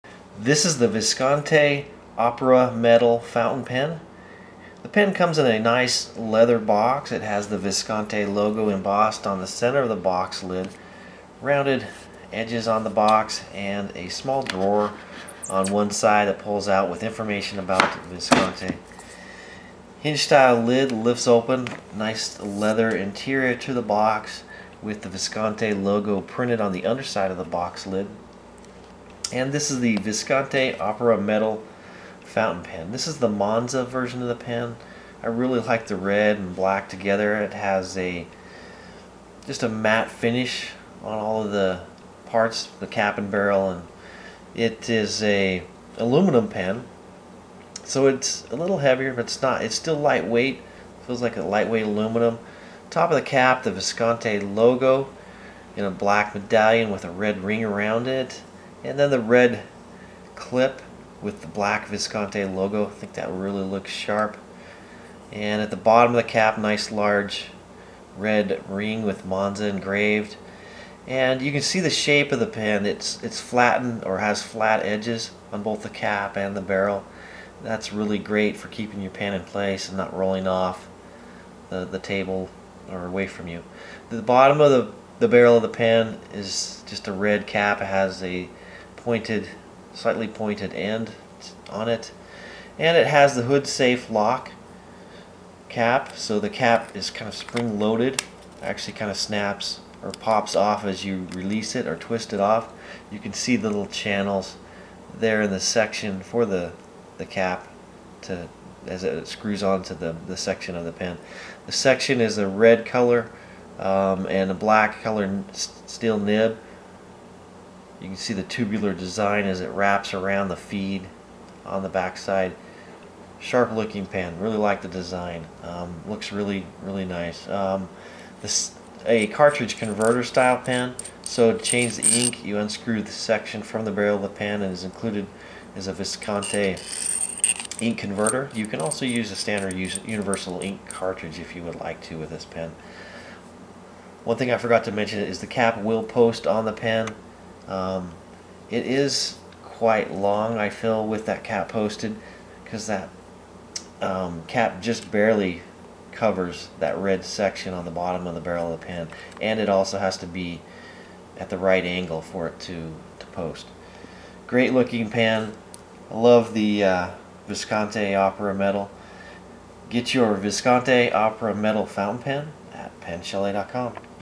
Visconti Opera Metal Fountain Pen
Details about the Visconti Opera Metal Fountain Pen

See the Visconti Opera Metal Fountain Pen up close. Learn how to change the ink. See what comes with the pen and how it is packaged. Get your Visconti Opera Metal Fountain Pen